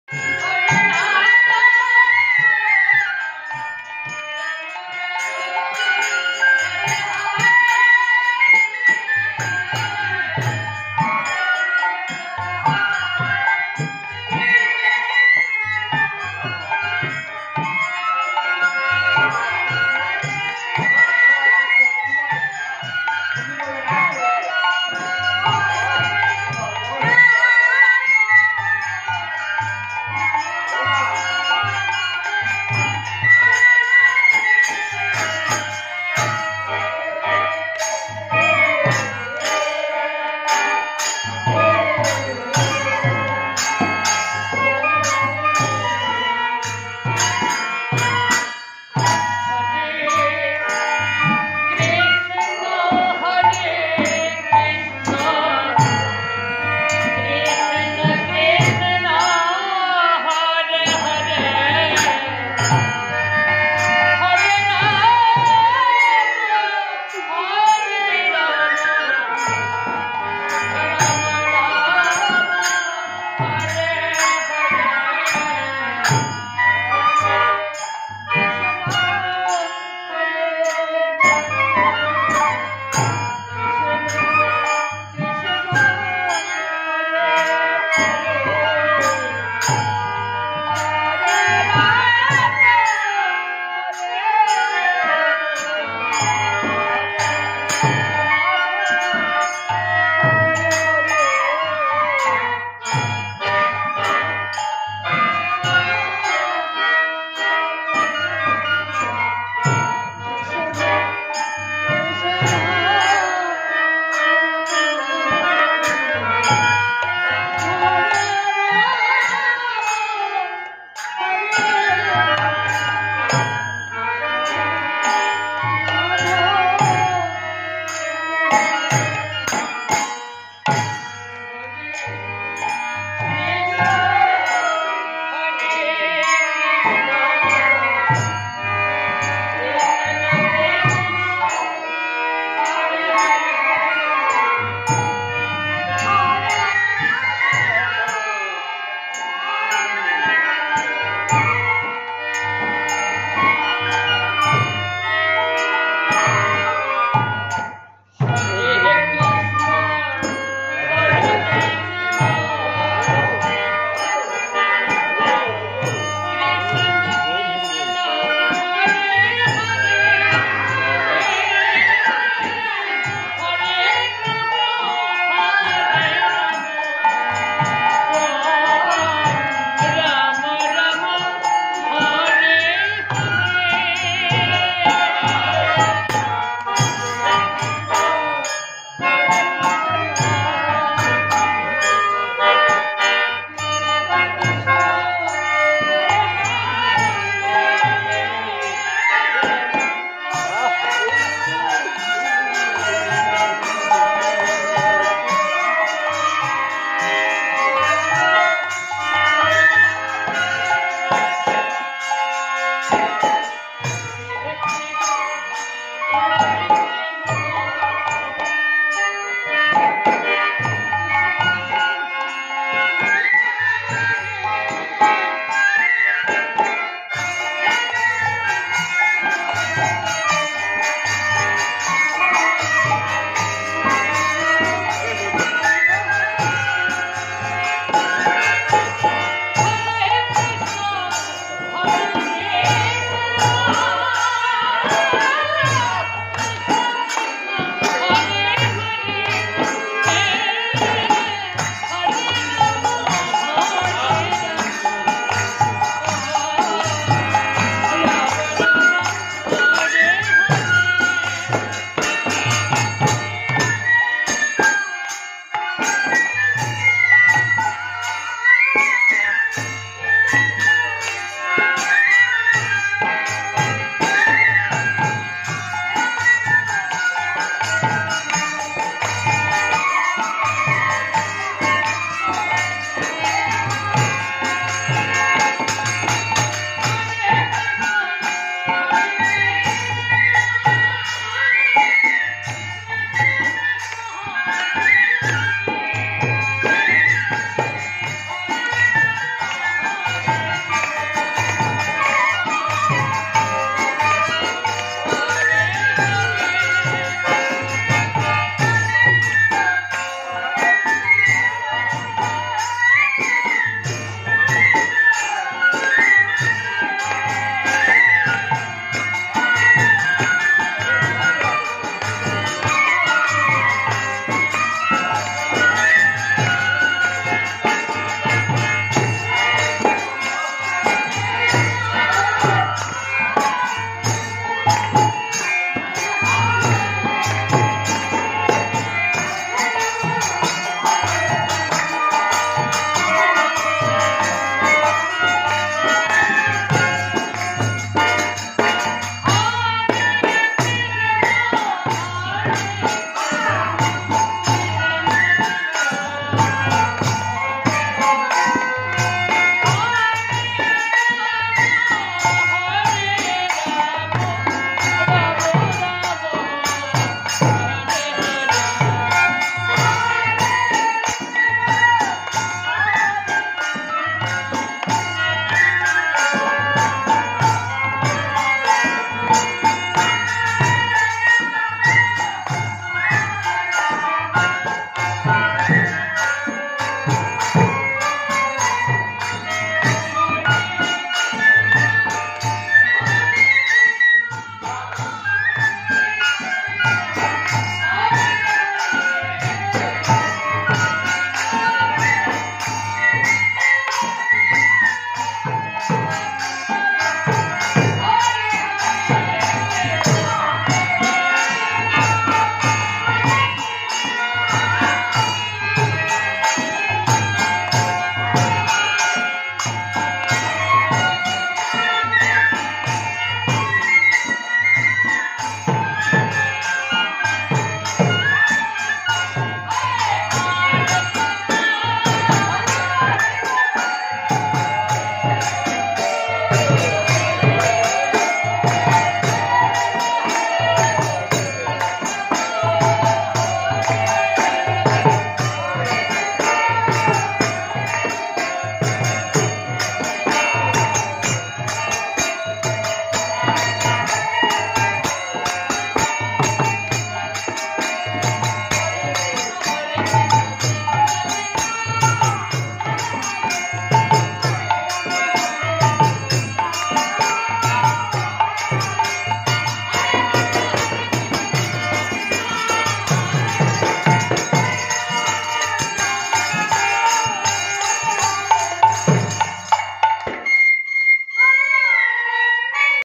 NABA DEPUSRI COMMUNITY(MADARIPUR)RASH NIGHT. 12AM2AM.9:3:22 KARNAFULI PEPAR MIL PRAKOLPO HARIMANDIR
Rangamati Kaptai Chandraghona Paper Mill Project Harimandi chanted Madhu's name
 24th Falgun 1429 Bengal
 09 March 2023 AD
 on wednesday

 Shree Nav Deepushree Community (Madaripur)
 Master: Deepu Joy Dhar
 Side: Sadhana, Lakshmi, Seethi
 Violin: Hemlal Dhar
 Twins:
 Srikhol : Anish, Liton
 Murali: Prashant Jay Dhar
 Cymbals: Sagar, Lytton
 Anger : Ras
 Asar time is 12:00 am to 02 am
 Directed by: Hemlal Jayadhar

 Charge to watch kirtan videos